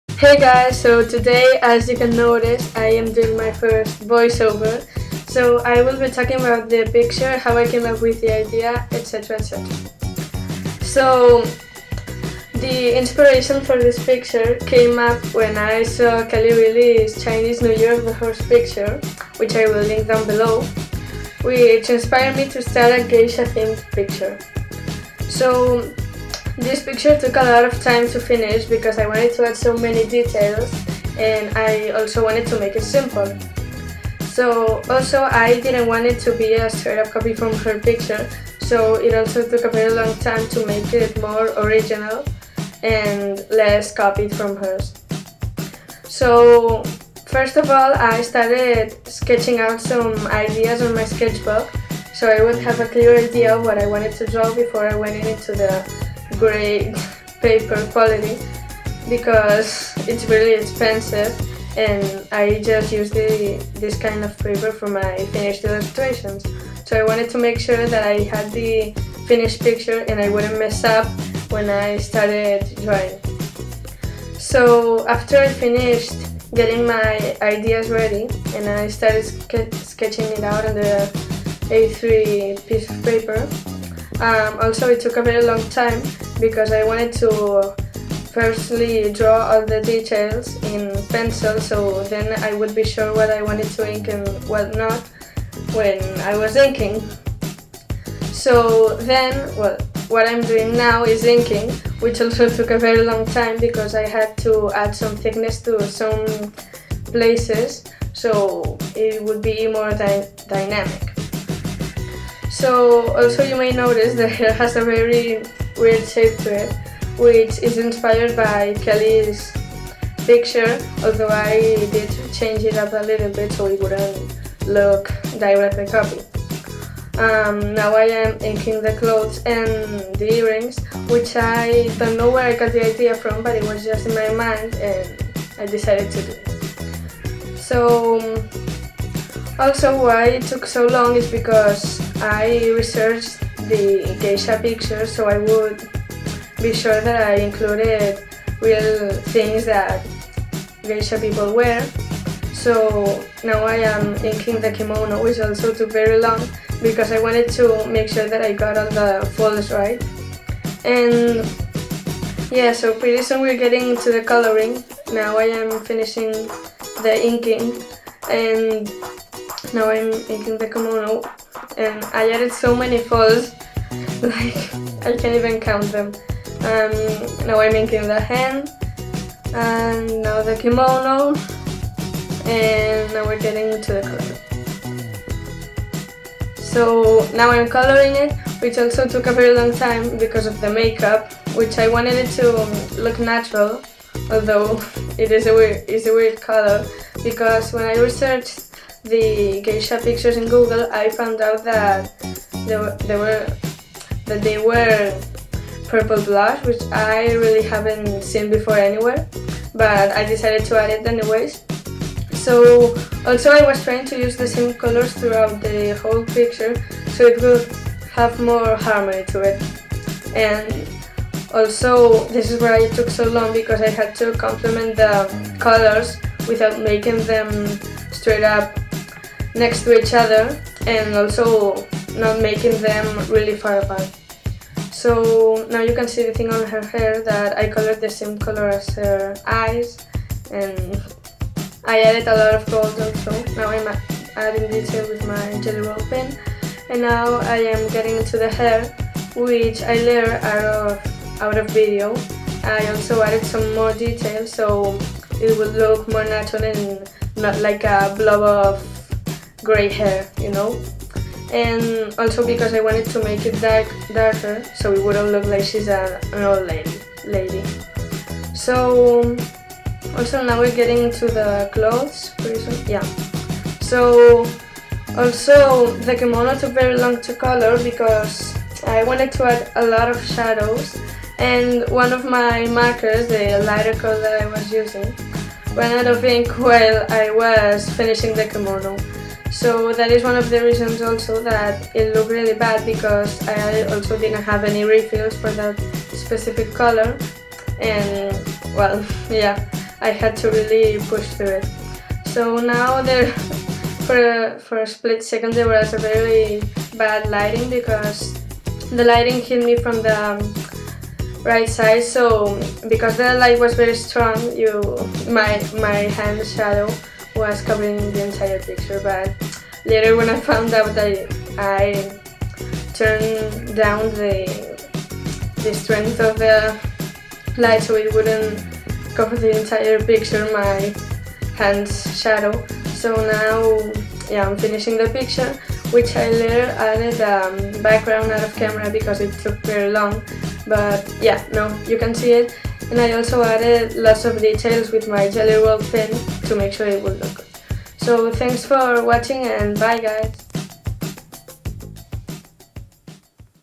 Geisha Girl [Speedink & Speedpaint]
Hey guys! So hey nothing important, just my FIRST VOICEOVER! So sorry for my Spanish accent, but I hope you liked the video! And if you didn't notice, this is the second video this week, and it's the second week in a row that I've done this! ^_^ YAY FOR ME!